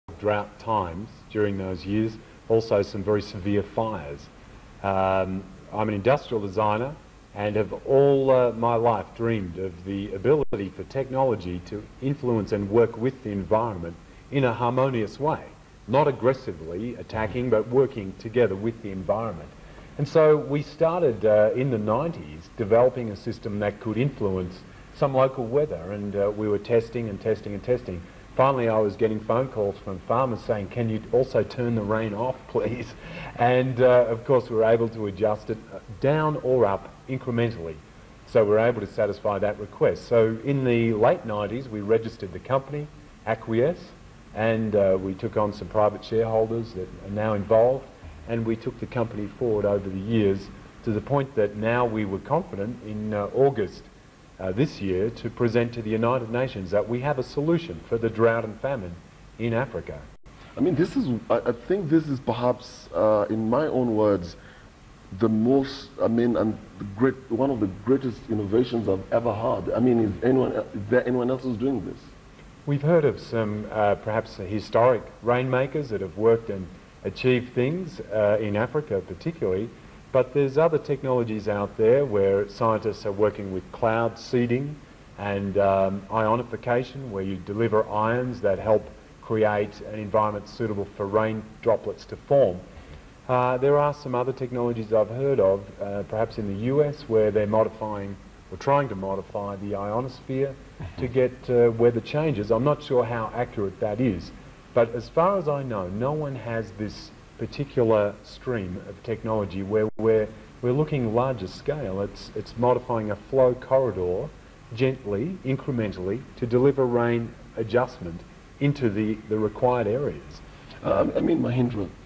aquiess KTN Interview How it works
aquiess Hi-Tech Rainmaking interview on Kenya's KTN 10 November 2011. KTN's Dennis Okari interviews Aquiess CEO David Miles and Director of International Planning and Communication, Dr Mahendra Shah. Prior to this interview Aquiess delivered a demonstration to bring early rainfall into the drought-declared region of the Horn Of Africa. The project to "Break the Drought" was announced in Rome on August 18th 2011, Transcript
In this statement Dr Shah says:
"We today undertake to deliver gentle rain within the next 45 days to break and end the drought and in particular extend the coming short rainy season."
Early rains came in and subsequent anomalous rainfall delivered better than expected results. The season was also extended beyond the normal short rainy season.

This is Clip-#3 from the KTN morning interview